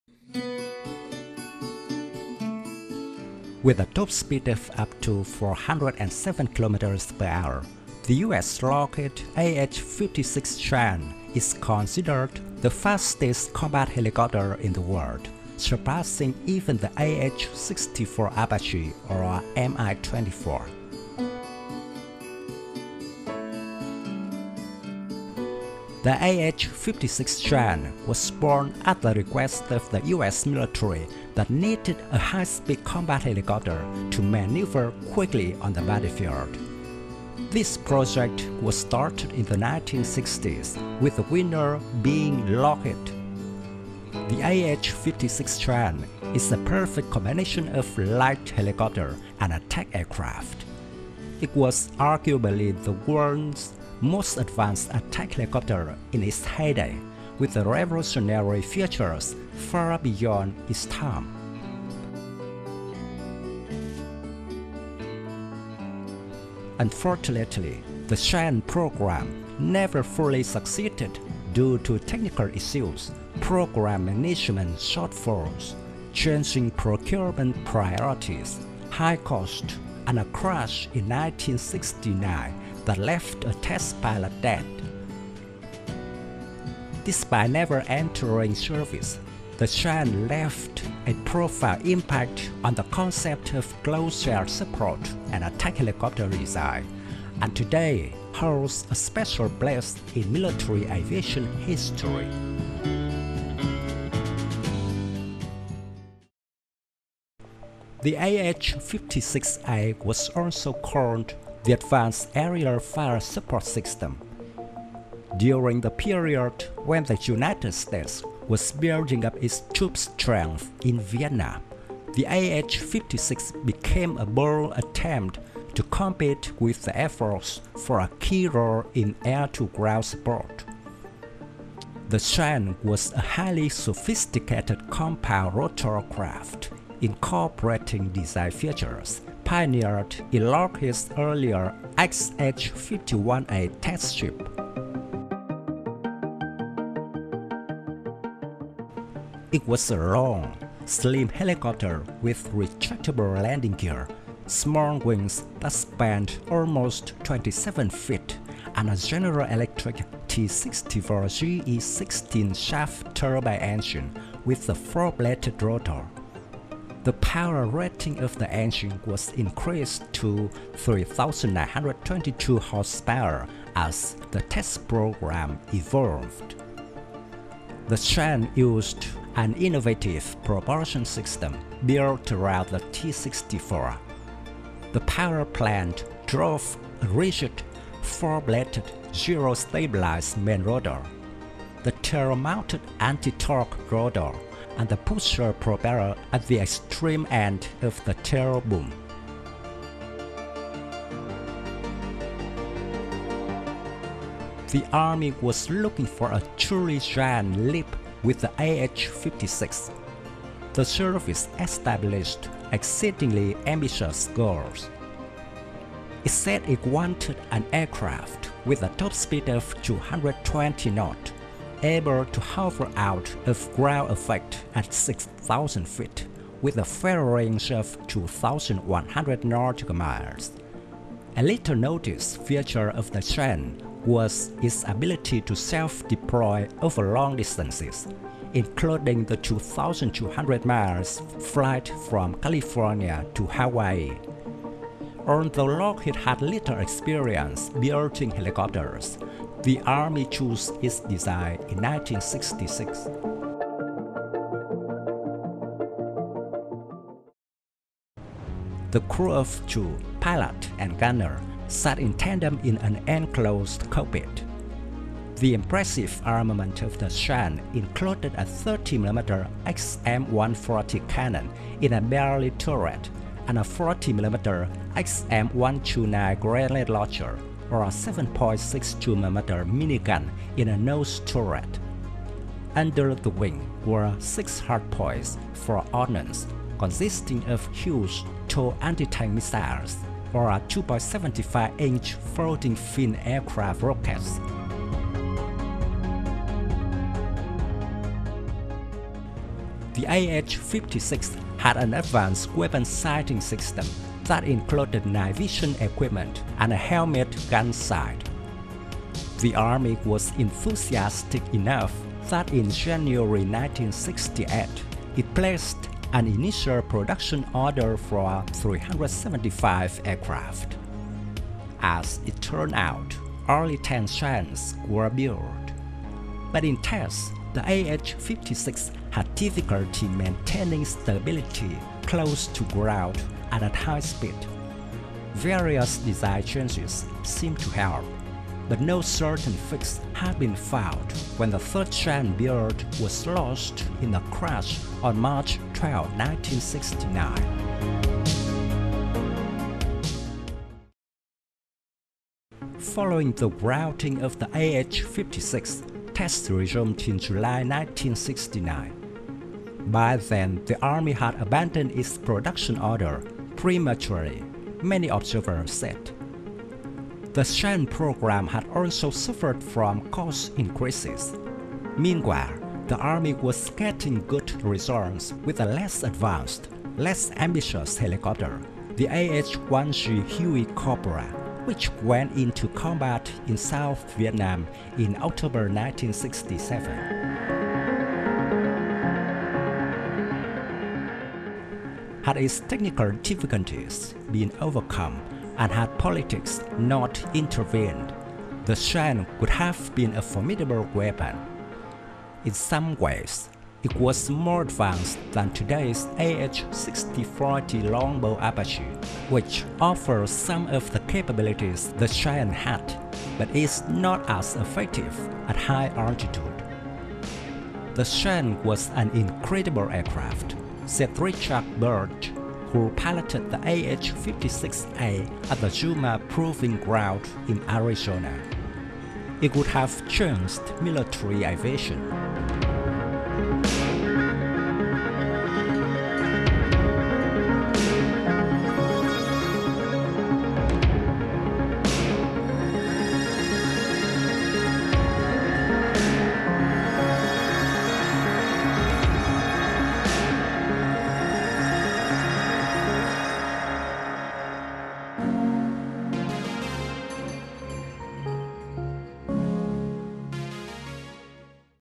AH-56 Cheyenne: Light Helicopter And Attack Aircraft In One
Lockheed AH-56 Cheyenne Is Considered An Incredible Attack Helicopter Decades Before Its Time. Packed with new technologies, the Lockheed AH-56 Cheyenne was designed to be an impressive attack helicopter ahead of its time. The Lockheed AH-56 Cheyenne attack helicopter was unique, reaching an amazing top speed of 245 miles per hour. Although it never made it past the prototype stages, the Cheyenne’s potential was obvious from the beginning. We will discuss the Lockheed AH-56 Cheyenne together in this video.